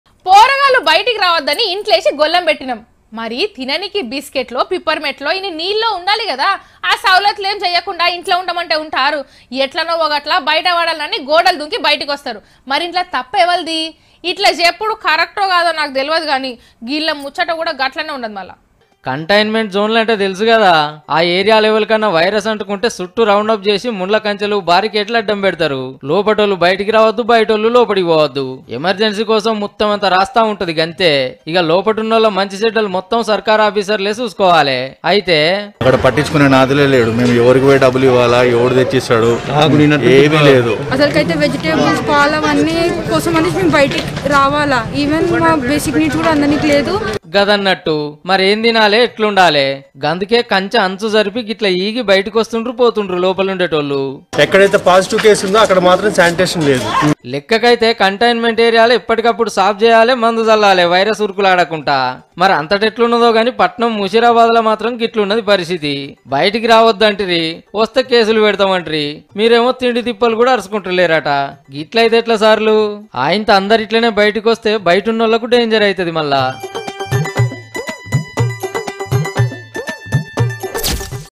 High Security in Coronavirus Red Zones, Public face problems to buy Essential Needs | V6 News
High Security in $Coronavirus RedZones, Public face problems to buy Essential Needs | V6 News